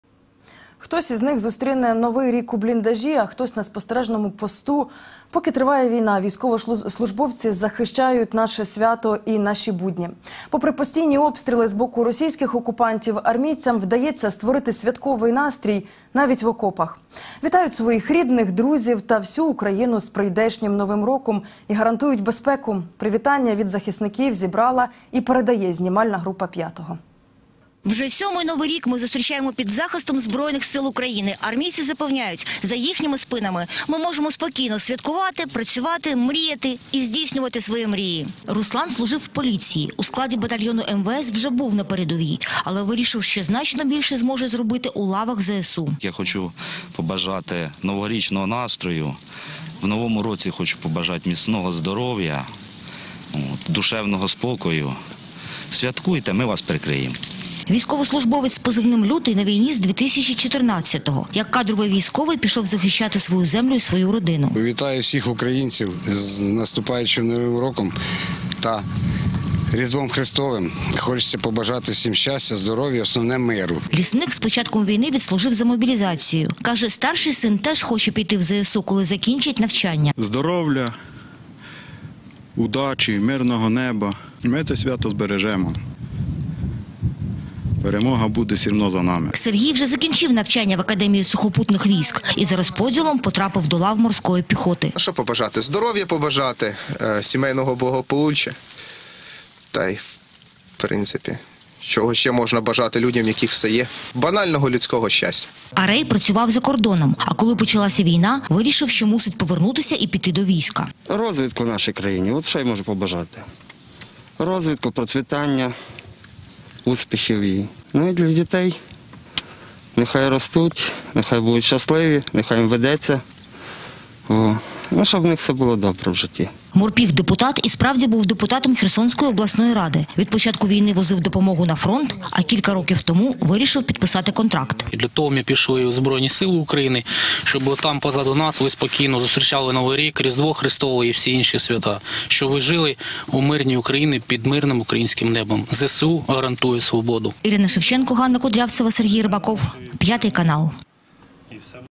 "Святкуйте, ми вас прикриємо": військові з передової вітають українців з Новим роком-2021
Хтось із них зустріне Новий рік у бліндажі, а хтось на спостережному посту. Поки триває війна, військовослужбовці захищають наше свято і наші будні. Попри постійні обстріли з боку російських окупантів - армійцям вдається створити святковий настрій навіть в окопах. Вітають своїх рідних, друзів та всю Україну з прийдешнім Новим роком і гарантують безпеку.